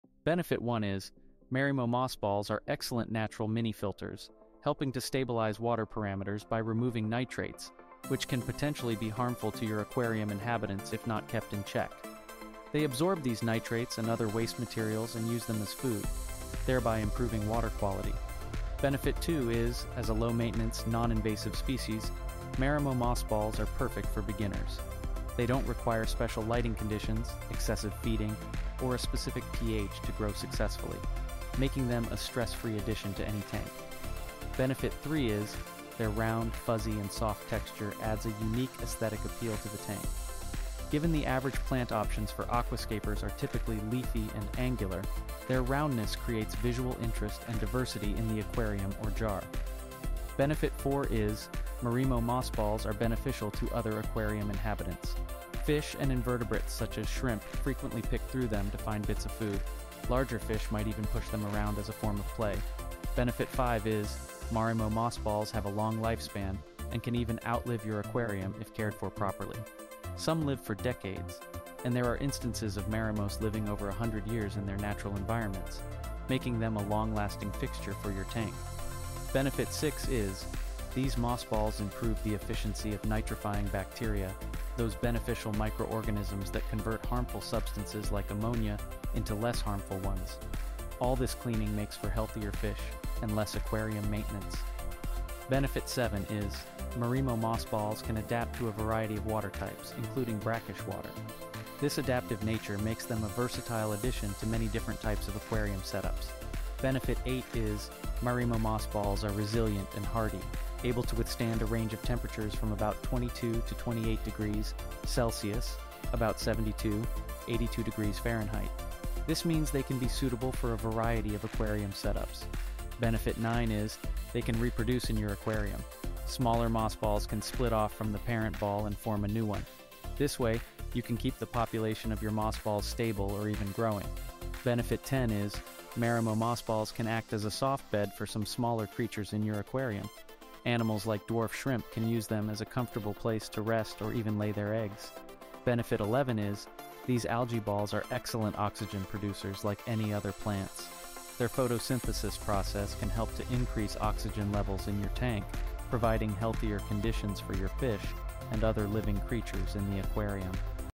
11 Wonders of Marimo Moss Ball
Discover the myriad benefits of introducing Marimo Moss Balls into your aquarium in this revealing video. Find out how these balls act as mini-filters, eliminating harmful nitrates and improving water quality. Ideal for beginners, these low-maintenance species do not require special conditions to flourish in your tank. Enhance the aesthetic appeal of your setup with their unique round, fuzzy texture. Learn how these fascinating organisms benefit other aquarium inhabitants and can even outlive your tank with suitable care. Delve into the roles they play in aiding nitrifying bacteria and adapting to various water types. Learn about their hardiness, reproductive abilities, function as a soft bed for smaller creatures, and their significant contribution to oxygen levels in your tank. Tune in and let Marimo Moss Balls transform your aquarium experience!